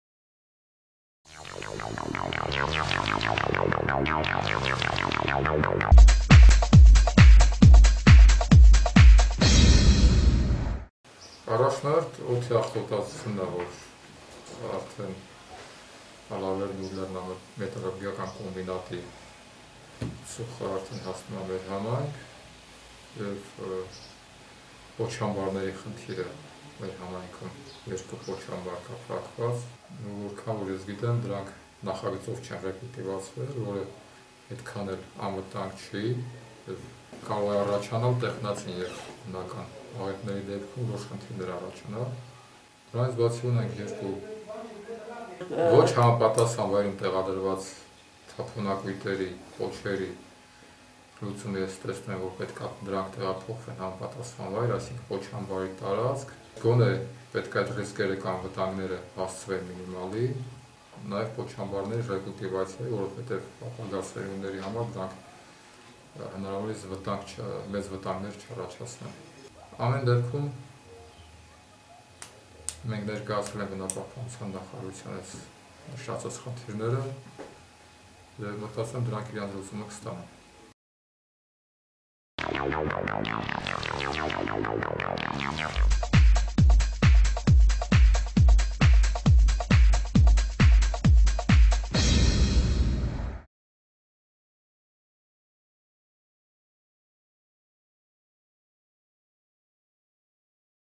Ախթալայի քաղաքապետը` էկոլոգիական խնդիրների մասին
Ախթալայի քաղաքապետ Հայկազ Խաչիկյանը ներկայացնում է Ախթալայի էկոլոգիական խնդիրները
We asked Akhtala Mayor Haykaz Khachikyan about the socio-environmental problems, and he answered briefly